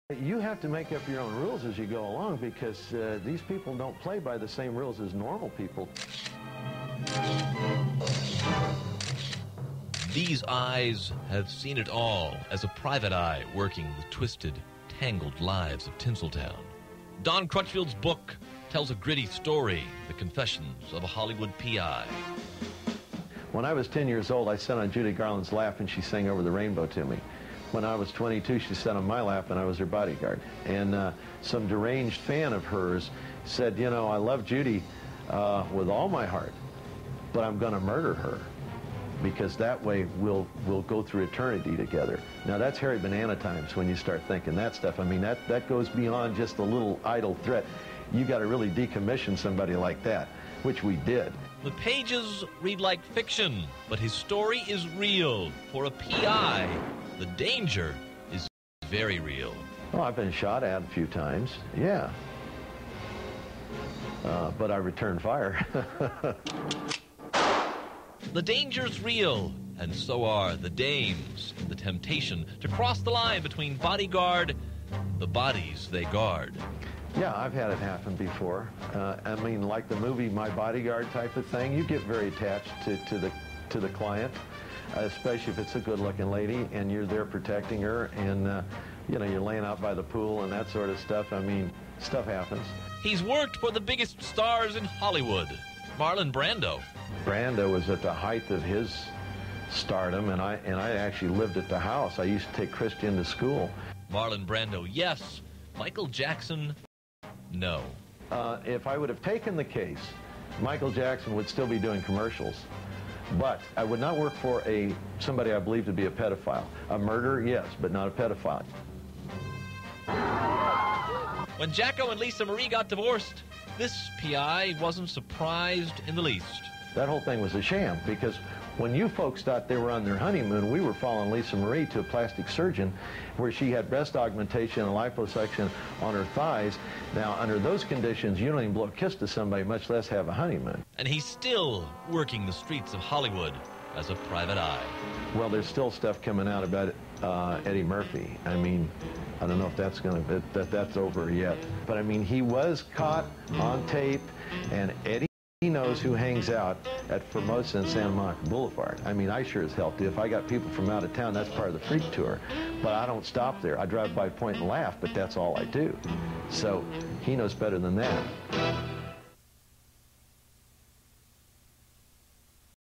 Don Crutchfield documentary of the dirty secrets of the hollywood elite
This video discusses the wild and exciting ride of Don's many PI uncovering's and the misgivings of the Hollywood elite in his Hollywood PI career.